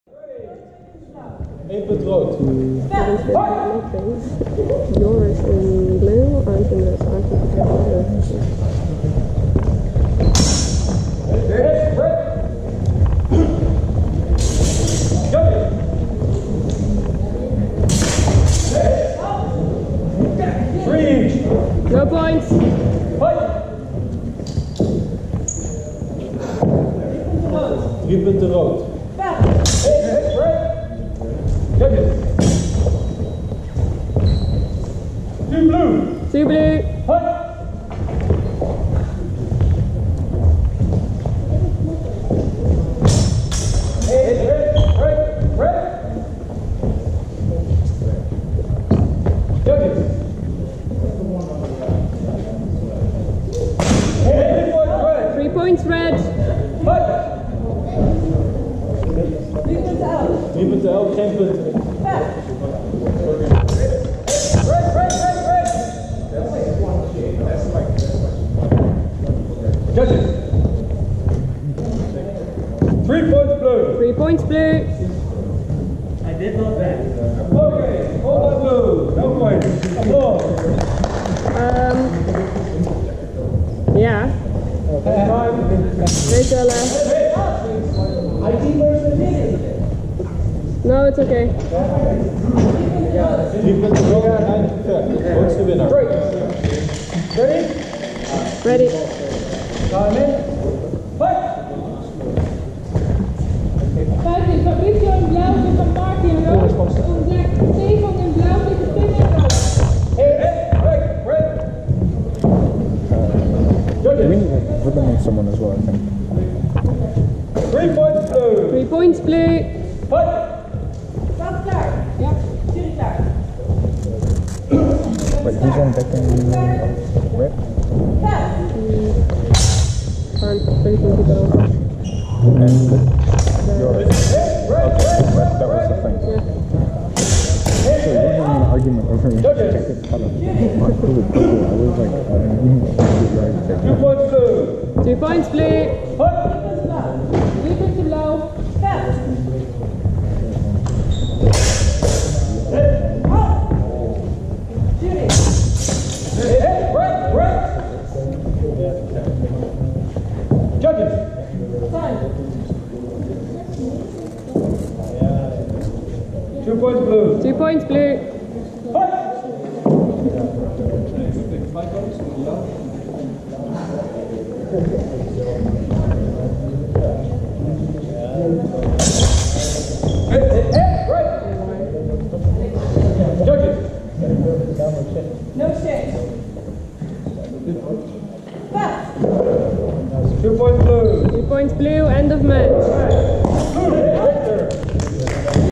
HVN Club Tournament '19 - Longsword - Arnd Koster vs Feiko de Vries
HVN Club Tournament 2019 - Longsword
Historisch Vrijvechten Nederland